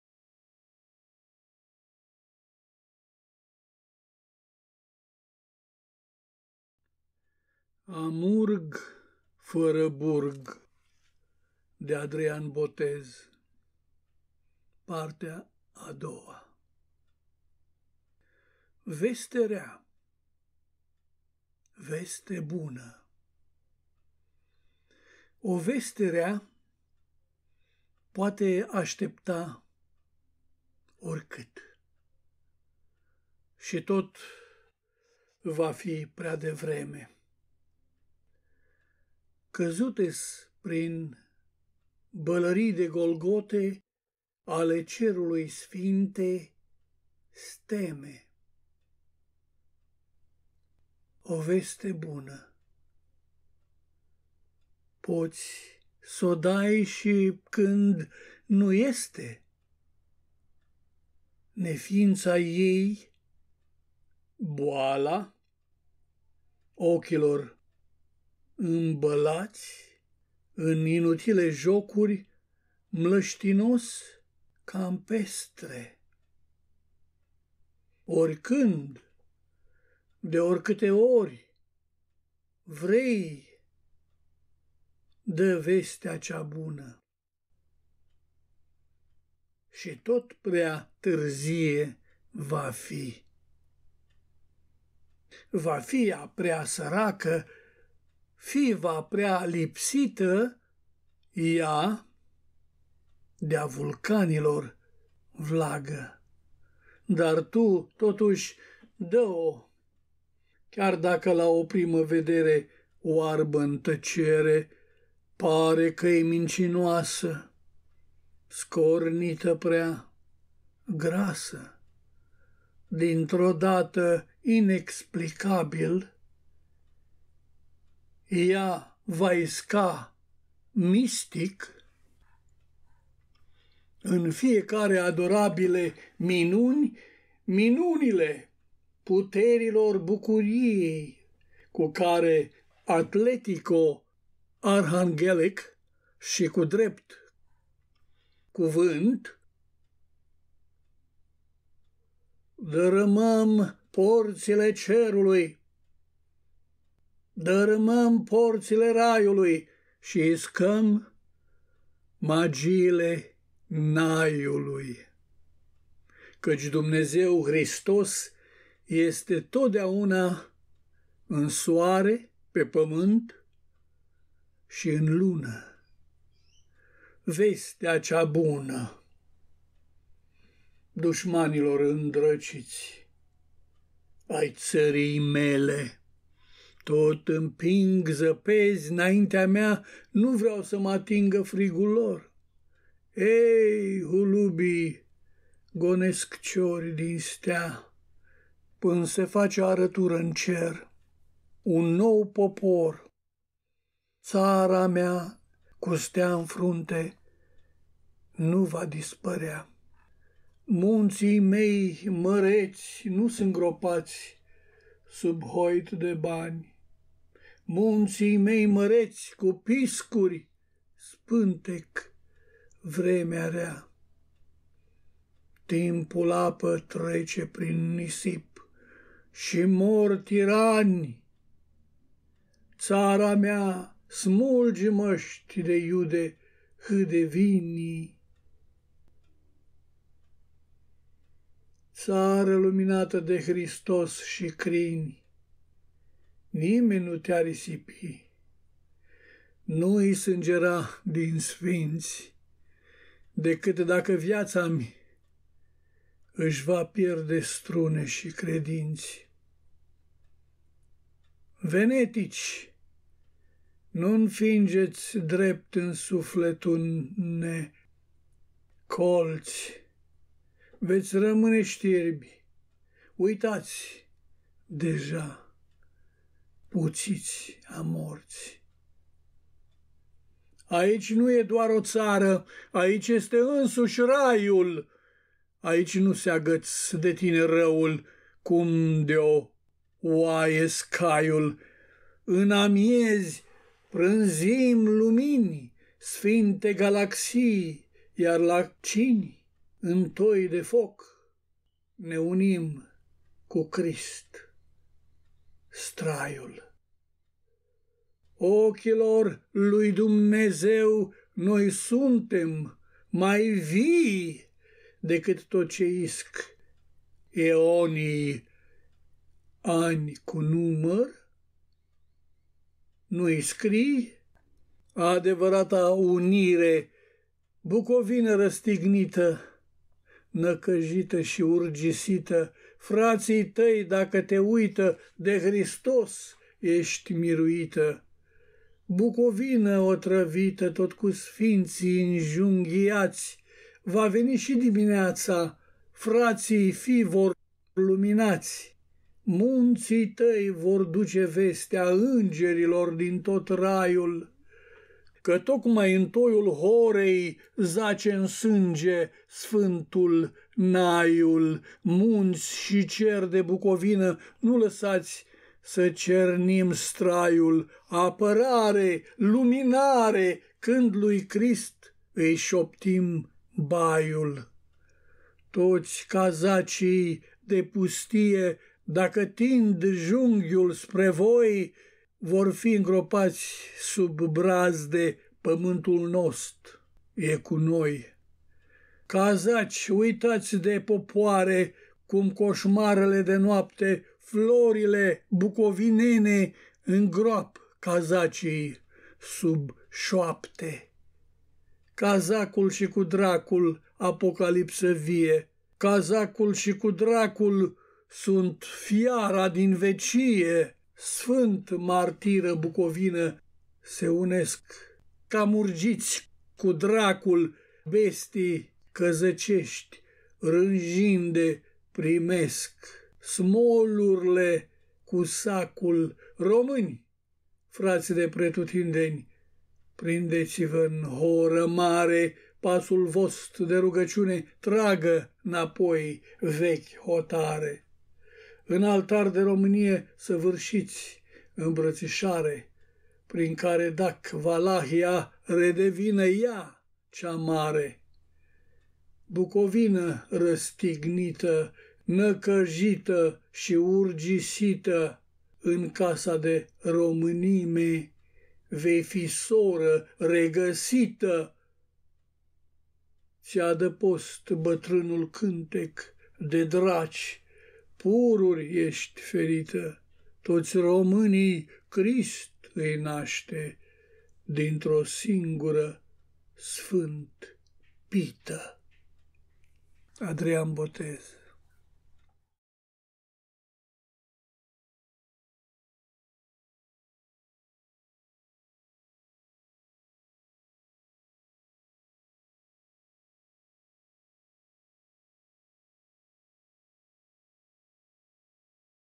AMURG FĂRĂ BURG II de Ben Todica, versuri ADRIAN BOTEZ
Un clip recitare la versurile poetului intitulat: AMURG FĂRĂ BURG de Adrian Botez -PARTEA A II-A- ~*~ VESTE  REA – VESTE  BUNĂ / O VESTE REA – poate / Aștepta – ORICÂT ! - ...și / TOT VA FI PREA DEVREME... / ...Căzute-s – prin / Bălării de Golgote  – ale Cerului SFINTE STEME...

O VESTE BUNĂ poți s-o
Dai - și când
Nu este (...Neființa ei : BOALA
OCHILOR ÎMBĂLAȚI - în Inutile Jocuri
Mlăștinos-Campestre !) : ORICÂND ! – de ORICÂTE ORI
Vrei – DĂ VESTEA CEA BUNĂ ! - ...ȘI TOT PREA
TÂRZIE VA FI ! – ...va fi ea prea
Săracă – fi-va prea lipsită - Ea - de-a
VULCANILOR VLAGĂ !
...Dar tu – totuși
DĂ-O ! – chiar dacă – la o
Primă Vedere (...Oarbă-n Tăcere...) – pare că-i
Mincinoasă – Scornită - prea
Grasă.. : dintr-odată - inexplicabil - ea
Va  isca – MISTIC !  - în Fiecare : ADORABILE
MINUNI !  -  MINUNILE
PUTERILOR BUCURIEI ! – cu care
(Atletico-Arhanghelic - și
Cu DREPT CUVÂNT !) – dărâmăm
PORȚILE RAIULUI - și iscăm
MAGIILE NAIULUI...
...Căci DUMNEZEU-HRISTOS  este – totdeauna
(în SOARE – pe PĂMÂNT și în
LUNĂ !) : VESTEA CEA
BUNĂ !

DUȘMANILOR ÎNDRĂCIȚI AI ȚĂRII MELE

Tot împing Zăpezi - 'naintea mea :
Nu vreau să m-atingă Frigul lor...
Ei - HULUBII – gonesc Ciori – din STEA
Pân' se face Arătură-n Cer : UN NOU POPOR !

Țara mea  - CU STEA ÎN FRUNTE – nu va dispărea :
Munții mei Măreți nu-s îngropați sub HOIT DE BANI !
Munții mei Măreți  - cu PISCURI  spintec'  Vremea Rea :
Timpul-Apă  trece  prin  Nisip - și  mor Tirani !

Țara mea : smulgi Măști de IUDE  - HÂDE VINI !
Țară luminată de HRISTOS și CRINI :
Nimeni nu te-a risipi  - NU-I SÂNGERA DIN SFINȚI

Decât dacă Viața-mi își va pierde STRUNE și CREDINȚI !
...VENETICI - nu-nfigeți (drept în Sufletu-ne !) – Colți :
Veți rămâne Știrbi – Uitați : DEJA – PUȚIȚI A MORȚI !
Aici nu e doar o Țară : AICI este ÎNSUȘI RAIUL !
Aici nu se-agaț' de tine Răul – cum de-o Oaie – SCAIUL !
În Amiezi – prânzim Lumini – SFINTE GALAXÌI
Iar la Cini – în TOI DE FOC – ne-unim - cu CRIST  – STRAIUL...

Ochilor Lui DUMNEZEU – noi suntèm mai VII
Decât tot ce isc' EONII : ”ANI CU NUMĂR” ? – nu-i SCRII !

ADEVĂRATA UNIRE

BUCOVINĂ  Răstignită – Năcăjită și-Urgisită :
Frații tăi dacă te uită – de HRISTOS ești miruită !
BUCOVINĂ Otrăvită  - tot cu Sfinții Înjunghiați
Va veni și DIMINEAȚA ! – Frații fi-vor  LUMINAȚI !

MUNȚII tăi vor duce VESTEA – ÎNGERILOR din tot RAIUL
Că – tocmai în TOIUL HOREI ! – zace-n Sânge – SFÂNTUL-NAIUL !
MUNȚI și CER de BUCOVINĂ – nu lăsați să cernim STRAIUL :
APĂRARE – LUMINARE – când Lui CRIST îi șoptim BAIUL !

Toți Cazacii-de-Pustie – dacă tind Junghiul spre voi
Vor fi îngropați sub Brazde : PĂMÂNTUL NOST' – E CU NOI !
Cazaci uitați de Popoare – cum Coșmarele de Noapte :
FLORILE BUCOVINENE îngroap' Cazacii – sub Șoapte !

(Cazacul și cu Dracul :
Apocalipsă Vie !
Sunt FIARA DIN VECIE !)

Sfânt-Martiră BUCOVINĂ – se-unesc AMURGIȚI – cu DRACUL !
Bestii Căzăcești – rânjinde – primesc SMOLUR'LE – CU SACUL !
Români ! – Frați de Pretutindeni : prindeți-vă-n HORĂ MARE !
Pasul vost' de RUGĂCIUNE – tragă  - 'napoi - VECHI HOTARE !

În ALTAR DE ROMÂNIE – săvârșiți ÎMBRĂȚIȘARE
Prin care DAC-VALAHÌA redevină EA – CEA MARE !
În  CASA DE ROMÂNIME – vei fi SORĂ REGĂSITĂ !

...Ți-e-Adăpost BĂTRÂNUL CÂNTEC :  de Draci  -  PURURI ești Ferită !
TOȚI ROMÂNII – CRIST îi naște dintr-o SINGURĂ SFÂNT-PITĂ !

Adrian Botez